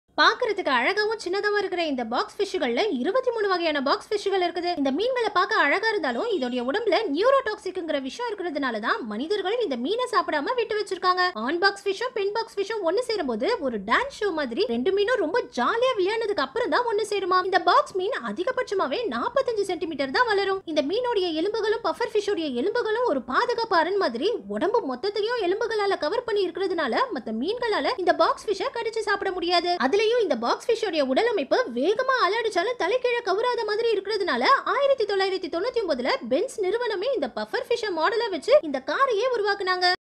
வித்தியாசமான மீன்|| Amazing Box Fish Facts || Tamil Info Share
This shorts about amazing box fish facts in Tamil